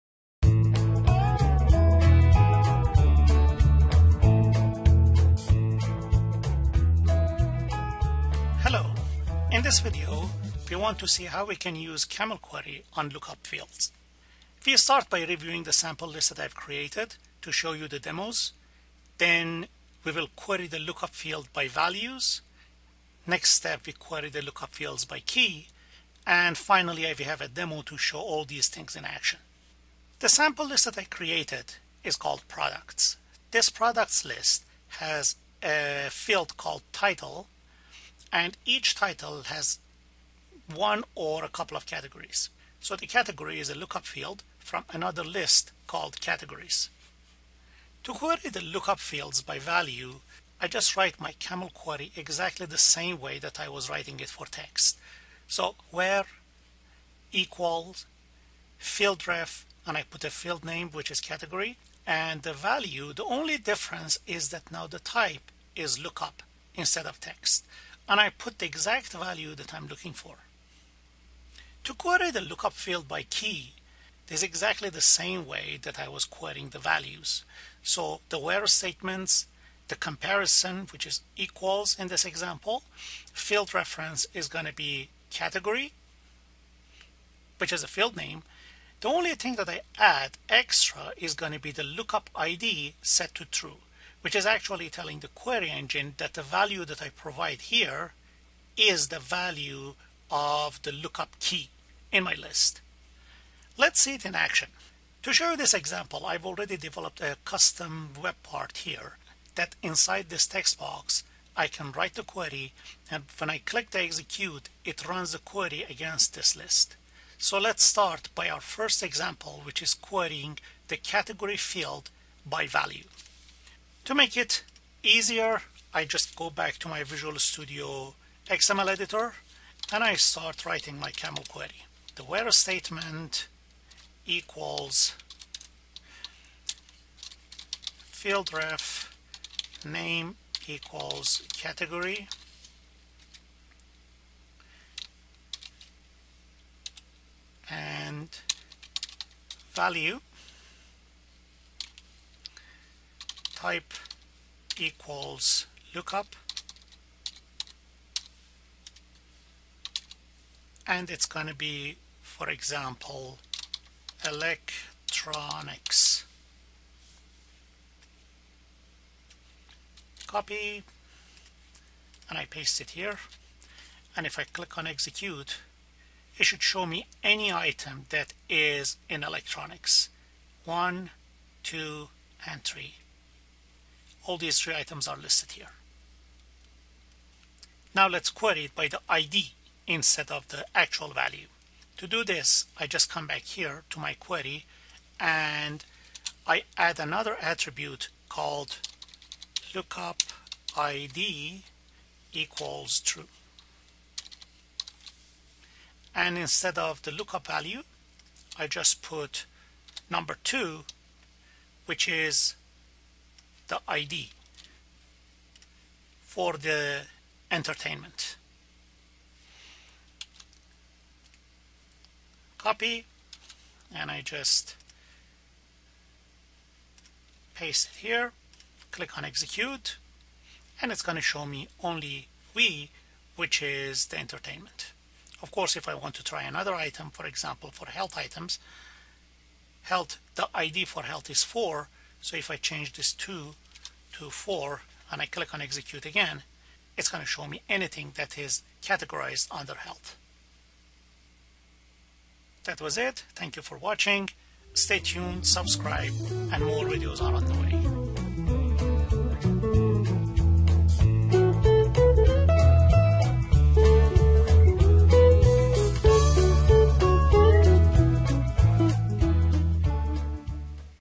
How to use CAML query on Lookup fields in SharePoint 2013
In this short video you will see how to write a SharePoint CAML query to filter the items  based on a lookup field.

To build your webpart that executes the CAML query refer to this video: